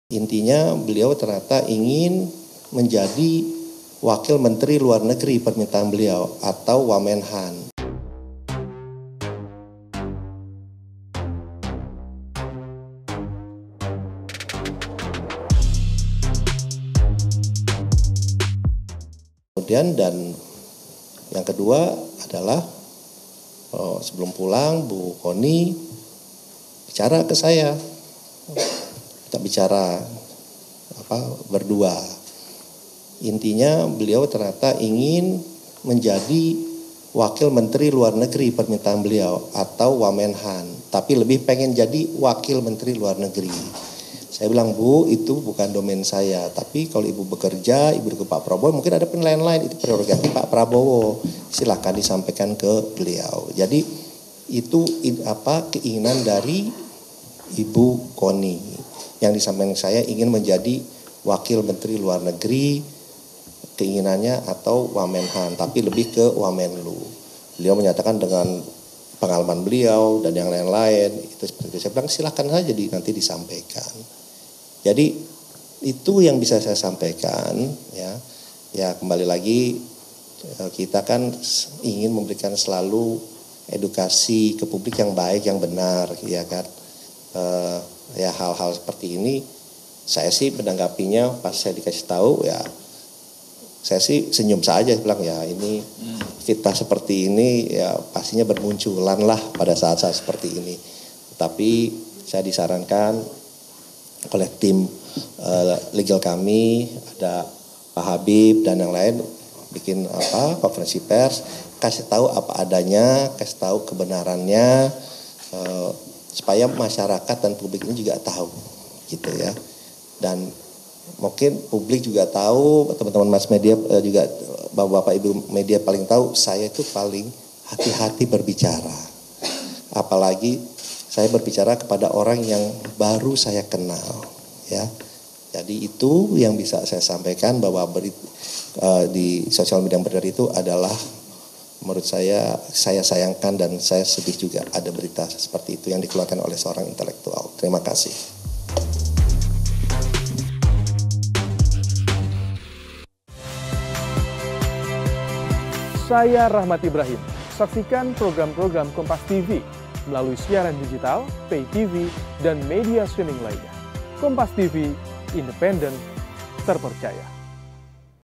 Ketua TKN Rosan Ungkap Connie Bakrie Minta Jabatan Wamenhan atau Wamenlu
KOMPAS. TV -  Ketua Tim Kampanye Nasional (TKN) Prabowo-Gibran, Rosan Roeslani ungkap Connie Bakrie meminta jabatan Wakil Menteri Pertahanan atau Wakil Menteri Luar Negeri. Hal ini disampaikan Rosan dalam konferensi pers pada Minggu (11/2/2024)

"Kebetulan ibu Connie bicara kepada saya, bicara berdua. Intinya ternyata beliau ingin menjadi wakil Menteri luar negeri, permintaan beliau atau Wamenhan. Tapi lebih ingin jadi wakil Menteri luar negeri," ujar Rosan.